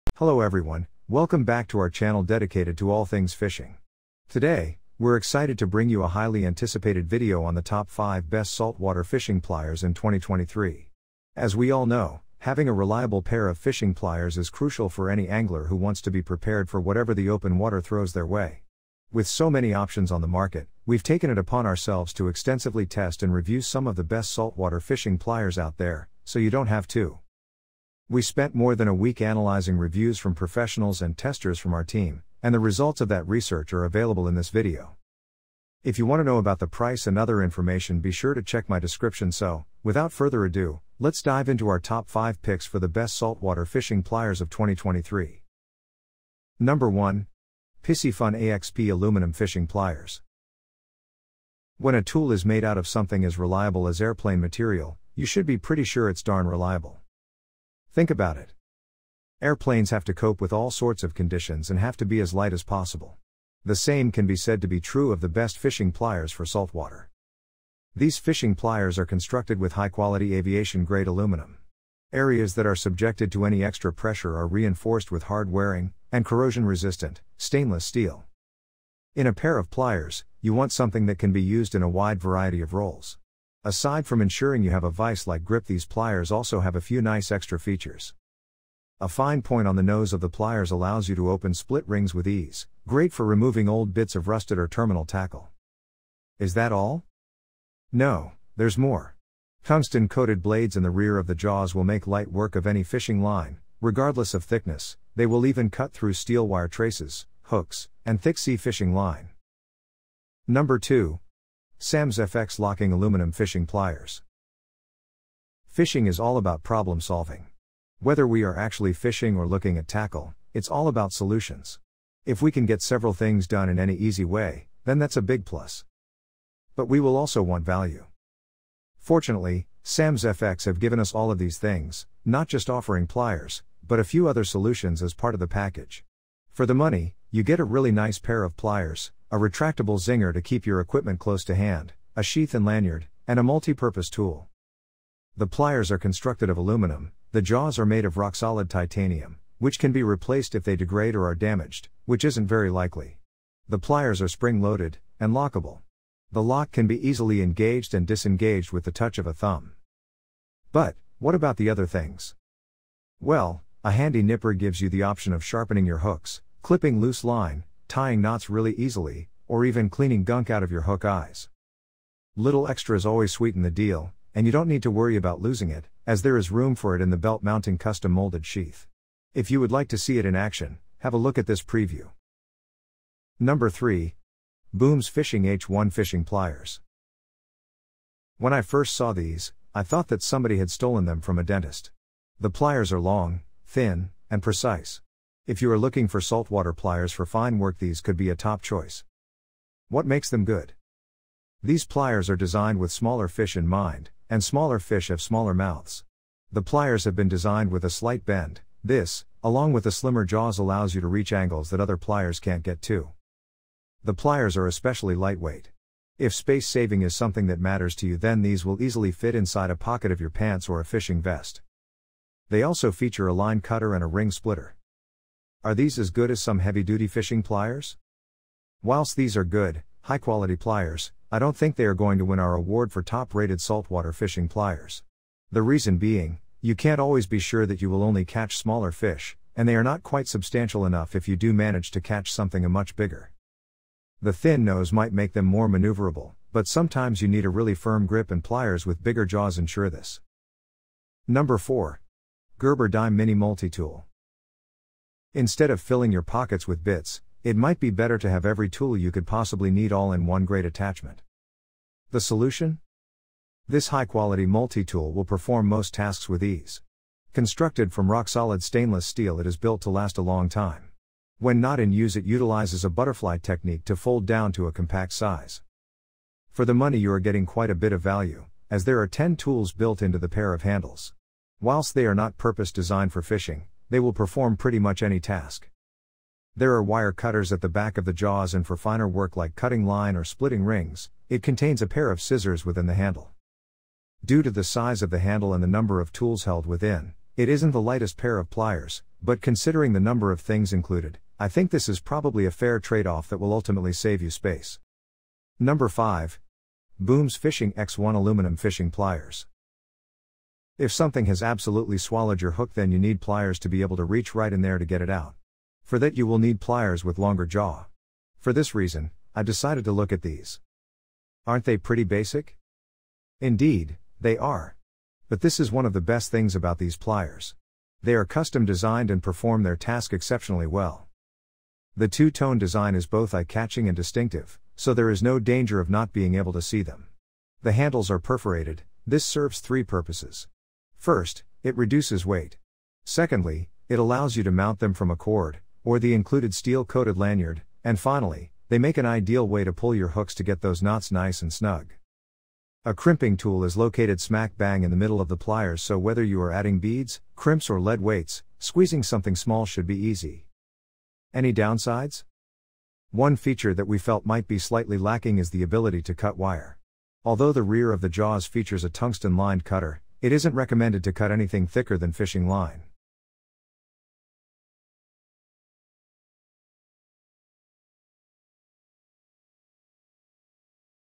Best Saltwater Fishing Pliers buying guide | Top 5 Saltwater Fishing Pliers
Are you looking for the best saltwater fishing pliers? Look no further! In this buying guide, we'll share with you the top 5 saltwater fishing pliers that you should consider buying.

From pliers for big game fishing to saltwater fishing pliers for smallmouth bass, we've got you covered. By the end of this buying guide, you'll have the perfect saltwater fishing pliers for your fishing needs!

✅ My Picks ✅1.Piscifun AXP Aluminum Fishing Pliers (Amazon)
✅ Best Value ✅2.SAMSFX Locking Aluminum Fishing Pliers (Amazon)
✅ For Beginners ✅3.Booms Fishing H1 Fishing Pliers (Amazon)
✅ Premium ✅4.Gerber Dime Mini Multi-Tool (Amazon)
✅ Trending ✅5.Booms Fishing X1 Aluminum Fishing Pliers (Amazon)